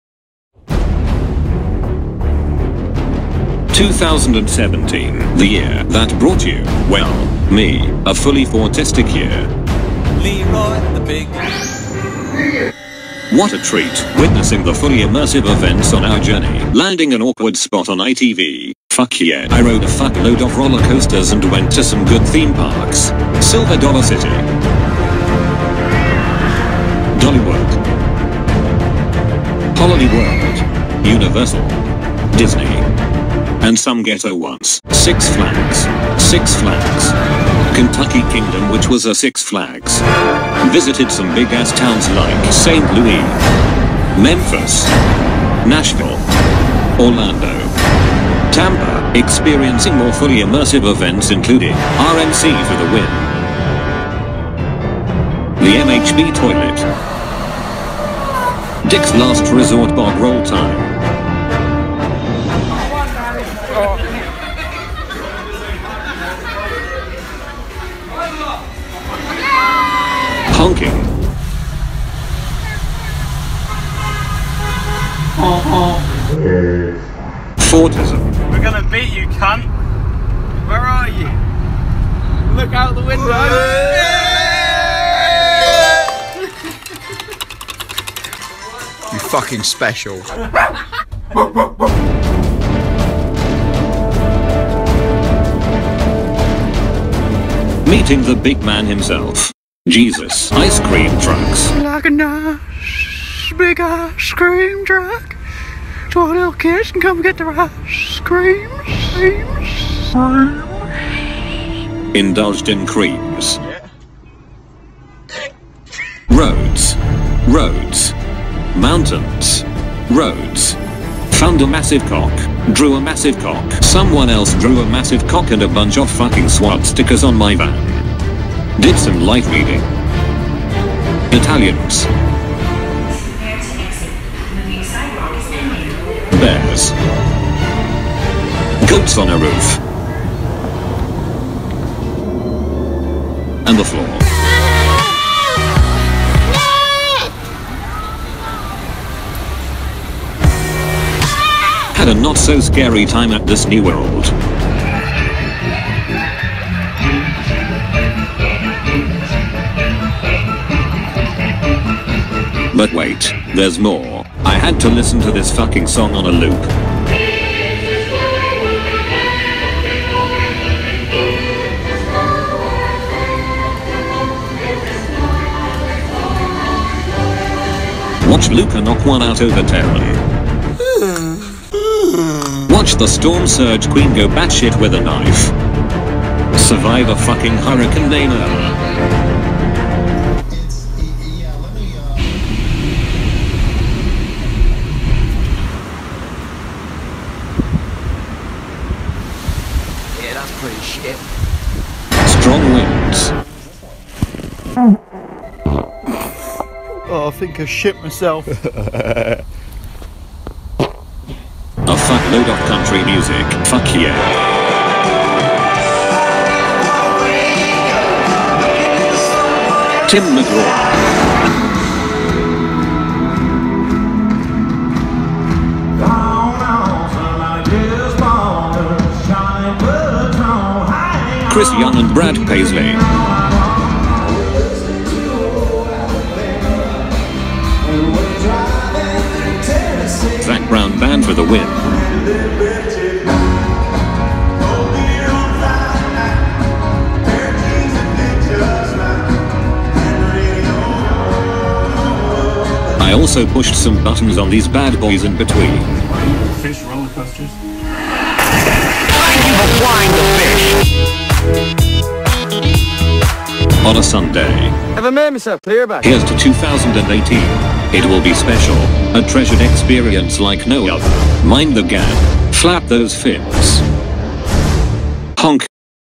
2017 - A YEAR OF FOURTISTIC ADVENTURES
This is my world of fourtism. From Traveling around America to anything else in-between with us, the Thorpe Park Staff. I take the piss out of a bunch of people so this will be... OFFENSIVE TO SNOWFLAKES.